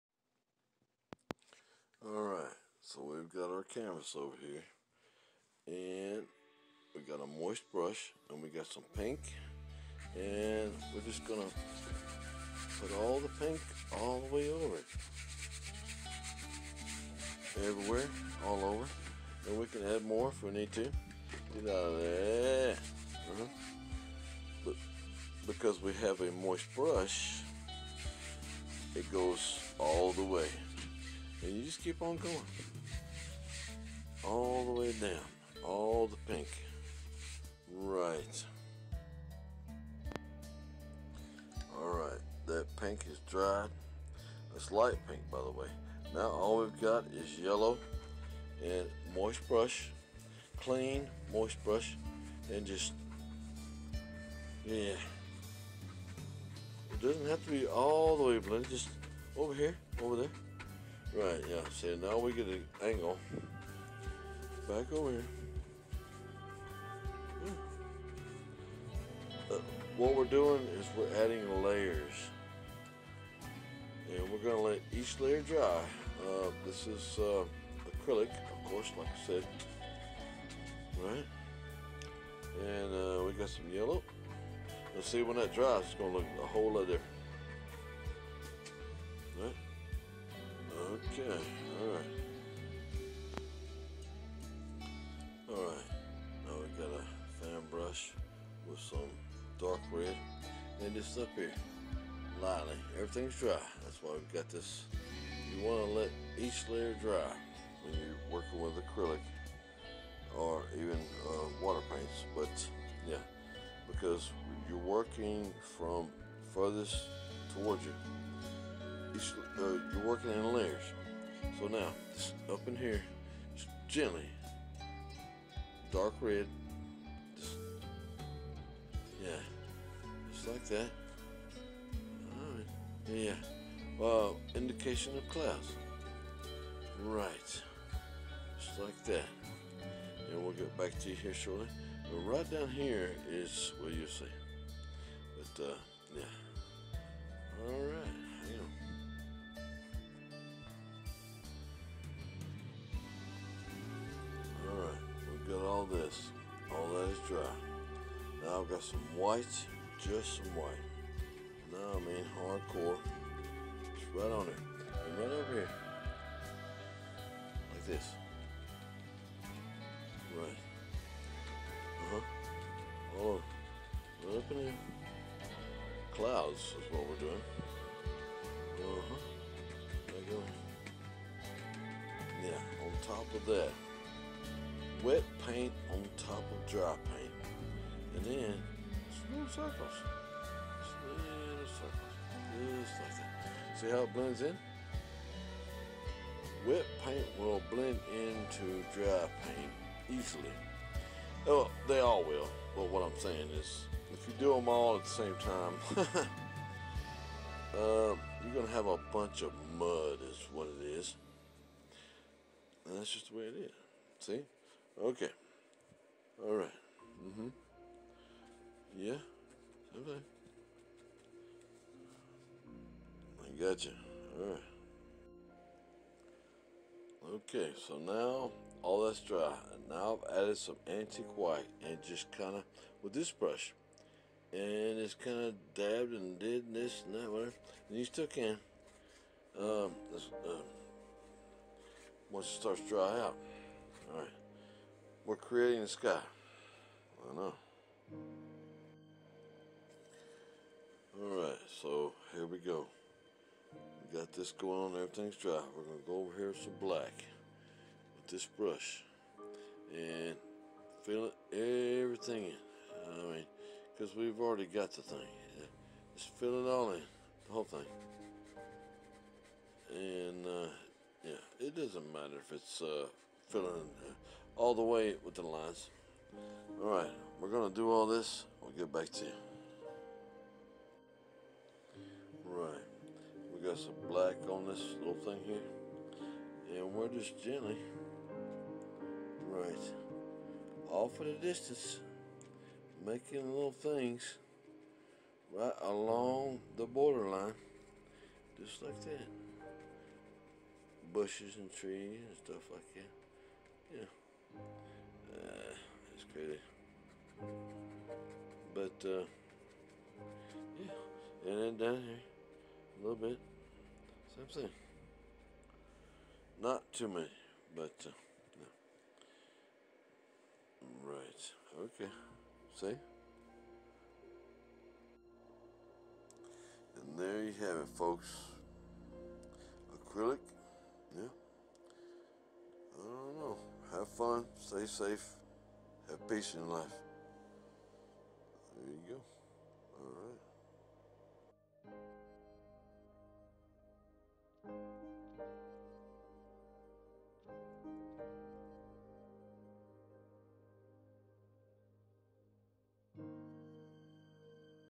Painting a magical nature scene in acrylic
Another fun and easy painting. *** Have fun, Peace and Joy in life
Geoff Harvey

Acrylic Paint Kit

Paint Brush Set